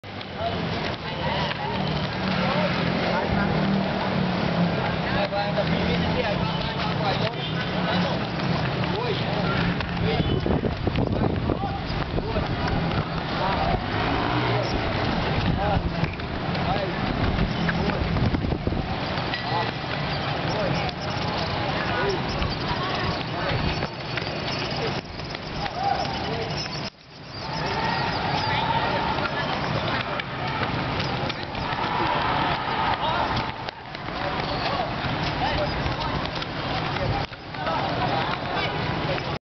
amateur football players in Phnom Penh
Some amateur football players playing, well, football in the Phnom Penh National Olympic Stadium in Phnom Penh, Cambodia. This is apparently where young men in Cambodia go every Sunday morning to have fun.